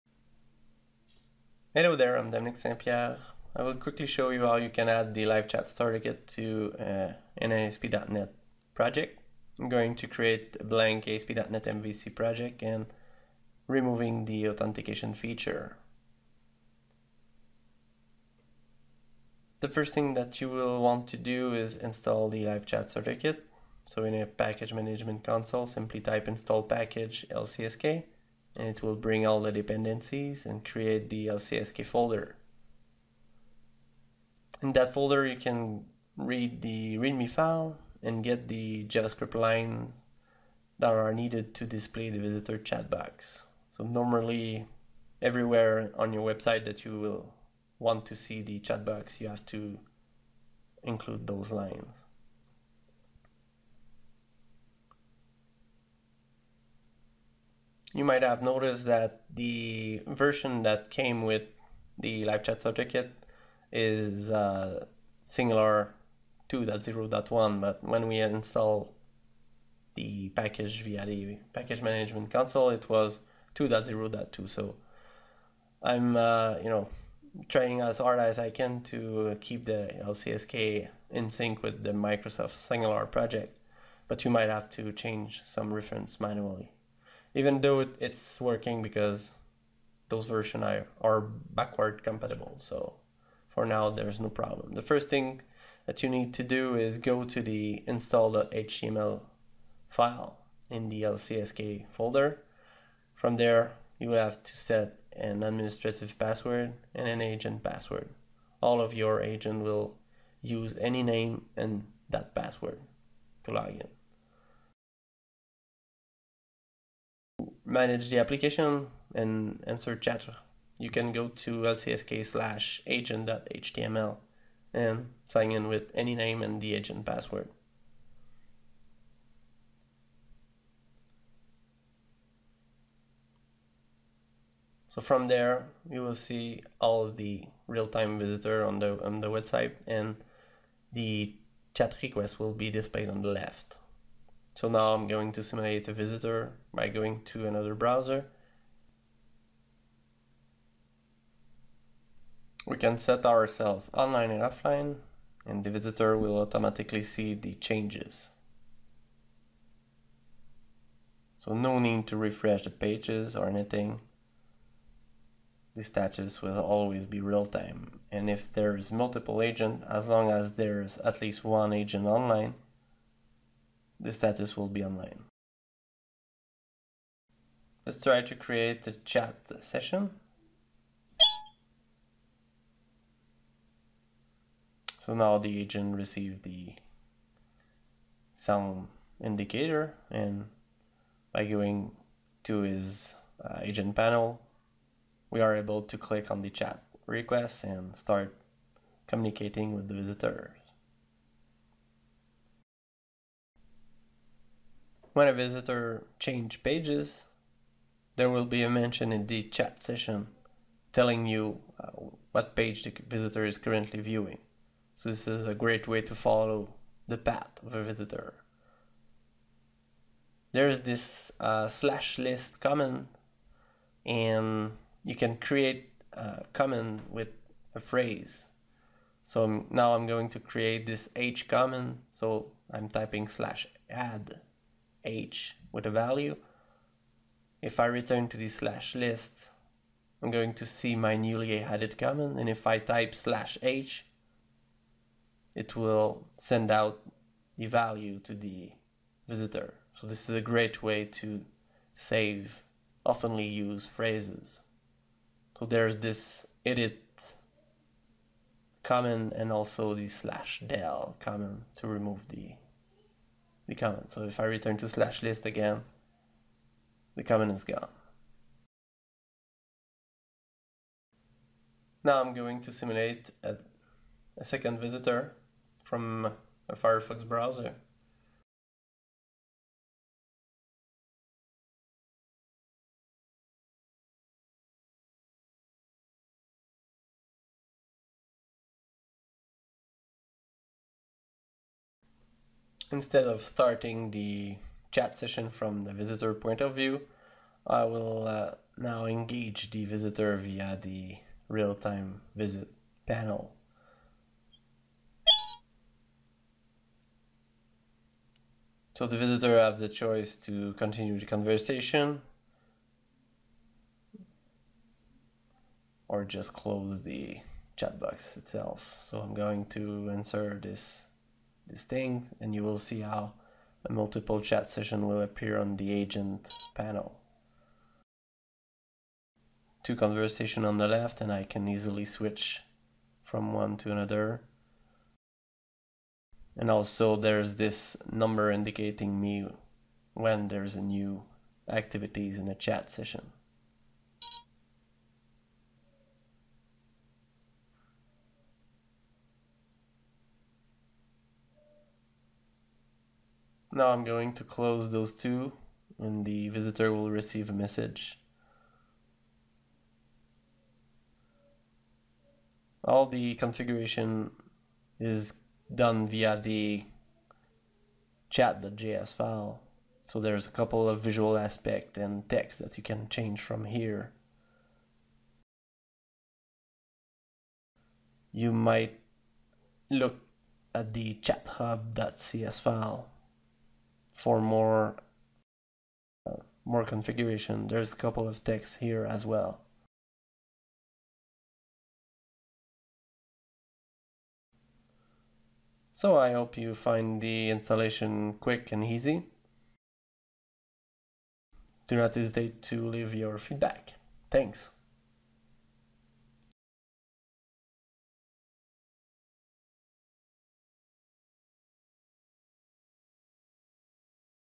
Add LiveChat Starter Kit to an ASP.NET projectt
I'm showing you how to add the LCSK 2.1.0 to an ASP.NET project